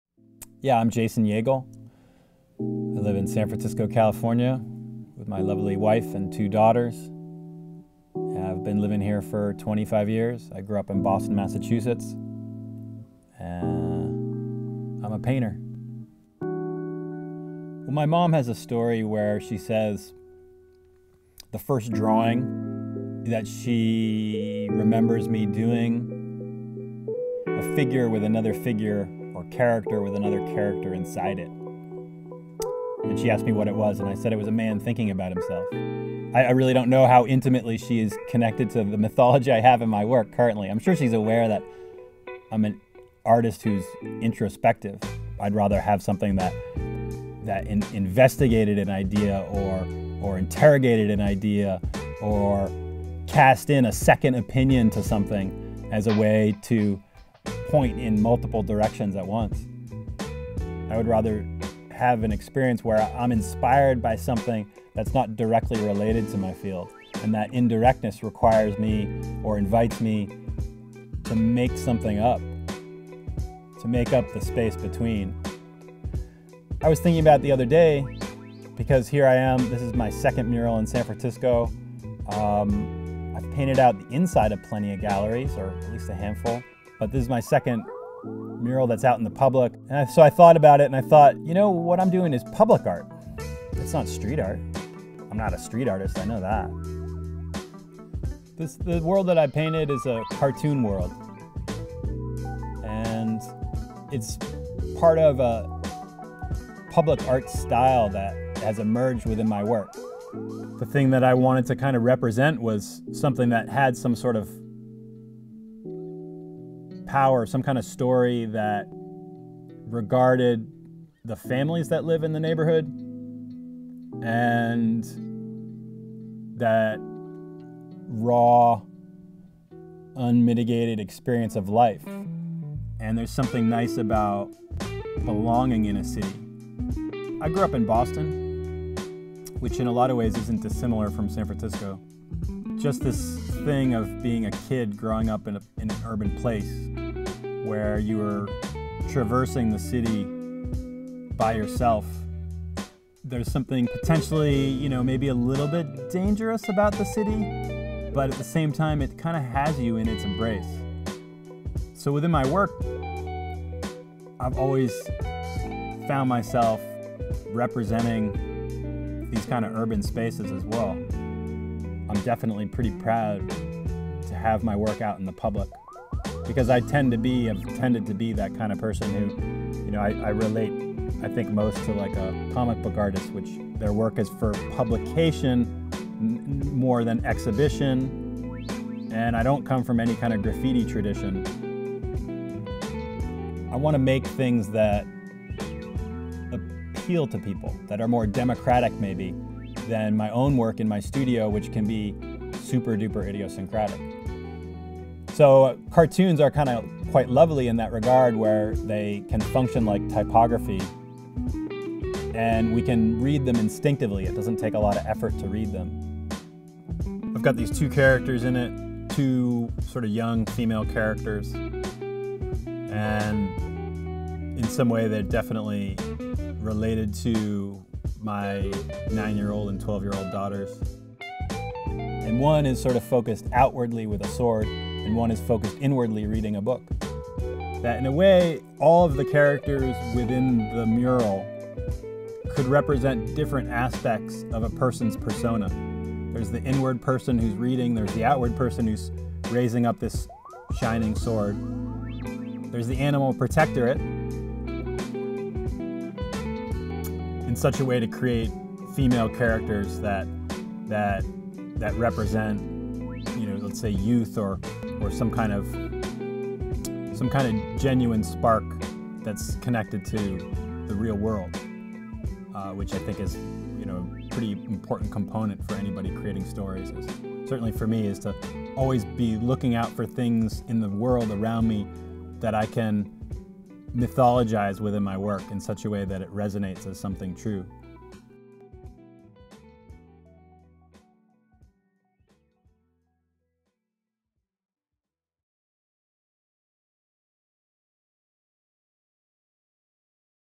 Jason Jagel the Painter
This was a cut I made on a project that was for film school. I think the school does. This is supposed to be apart of my editing portfolio. Enjoy